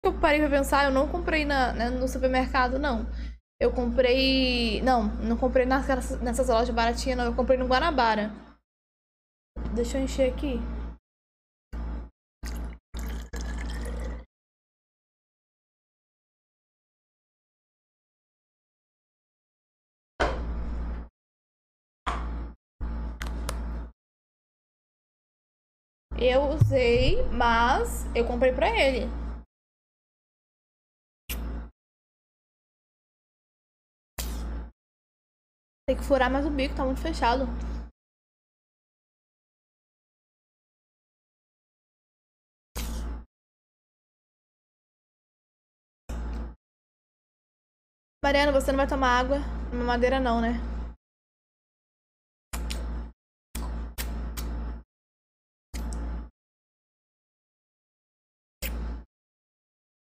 MARI MENTIU SOBRE O GORDIN GOSTAR DE MAMADEIRA? A MAMADEIRA É DELA?! | Cortes da Marifps
• Acompanhe a Mari ➤ Procure por "marifpss" no face.

➥Instagram 📸: @mariifps
➥Twitter 🐦: @marifpss